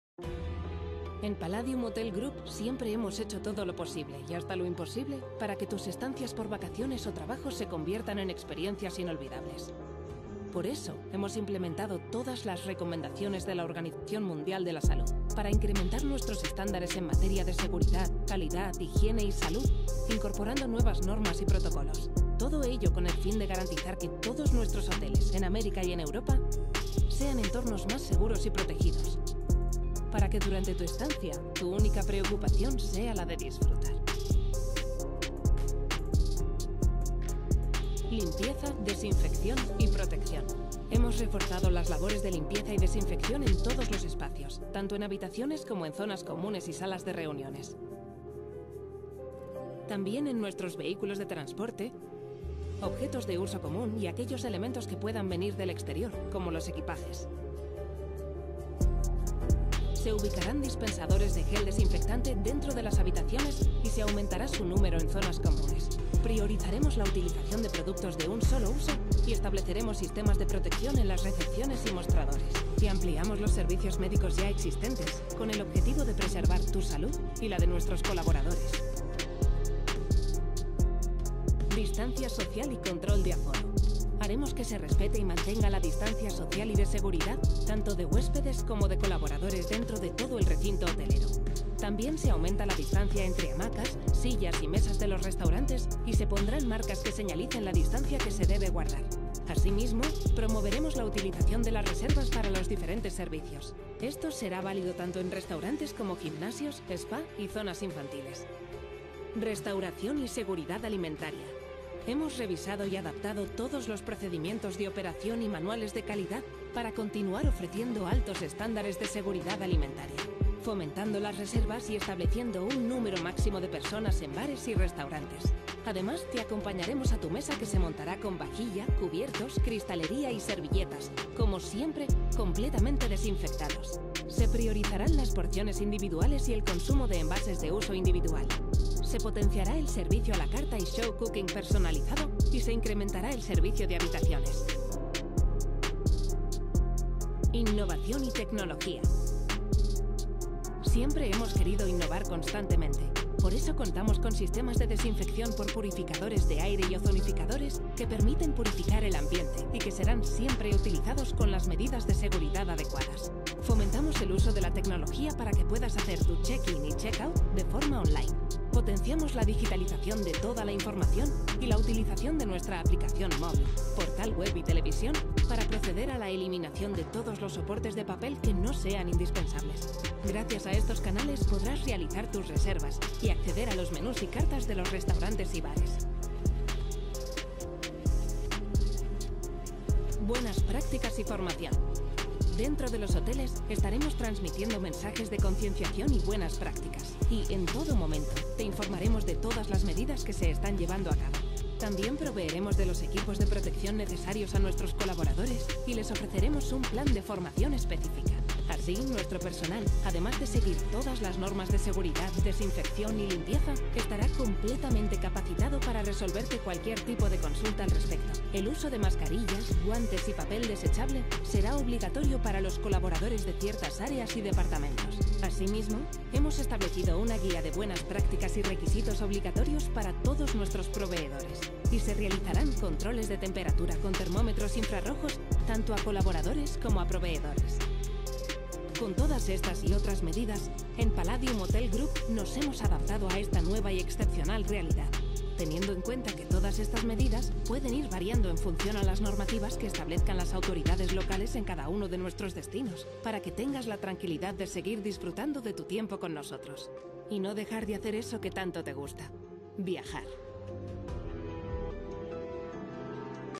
► Grand Palladium Punta Cana ◄ ▲ Protocolo para REAPERTURA ▼ [Punta Cana todo incluido 5 estrellas ]
El Grand Palladium Punta Cana nos permite ver como sera el protocolo de reapertura, como se usaran los restaurantes del grand palladium punta cana, las habitaciones tendrán un mayor servicio .

Grand Palladium Punta Cana Restaurantes
Grand Palladium Punta Cana Bares
Grand Palladium Punta Cana Piscinas
Grand Palladium Punta Cana Habitaciones